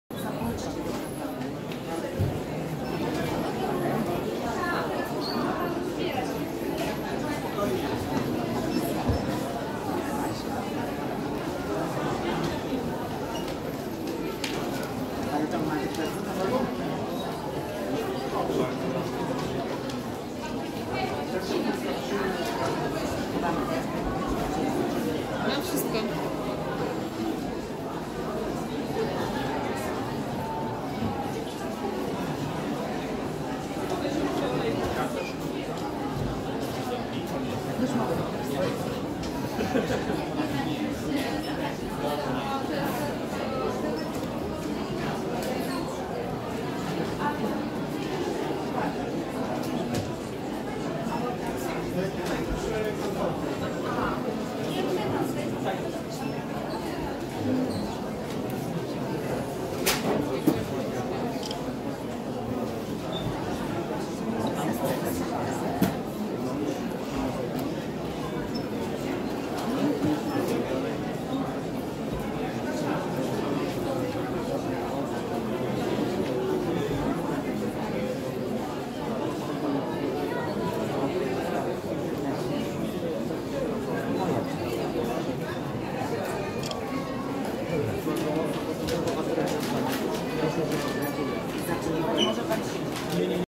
Warszawskie Targi Książki 2019 (Katarzyna Grochola)
Kilka słów wpisu, do zakupionej przez Panią Katarzynę Grocholę "Słuchajcie siebie".
Warszawskie Targi Książki 2019.